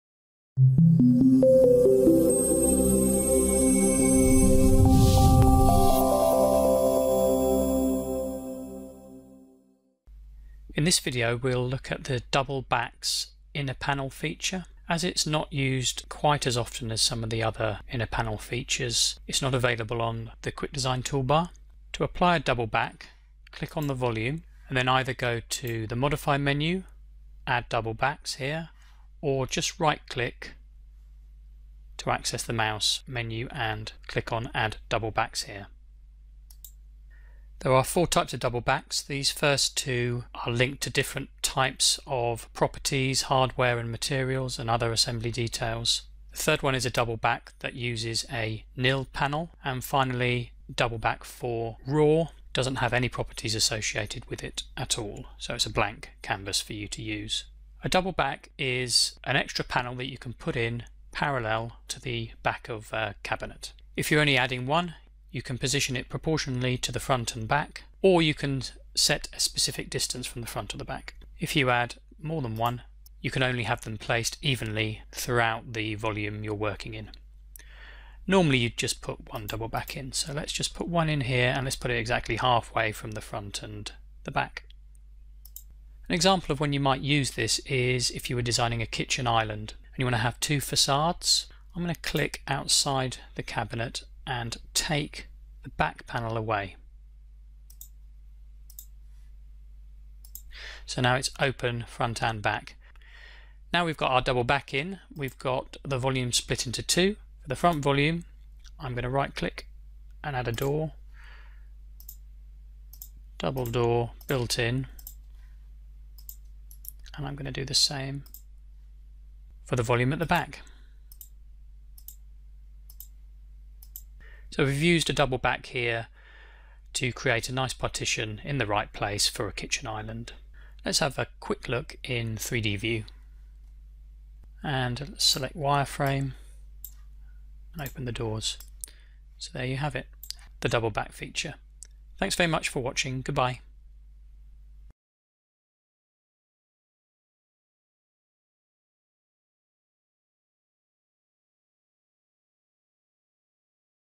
Double Backs in Polyboard
These double backs are simply panels positioned parallel to the back but they can be brought forward to improve design flexibility. One application is to split the cabinet front to back and add doors or drawers to both, as in a kitchen island cabinet.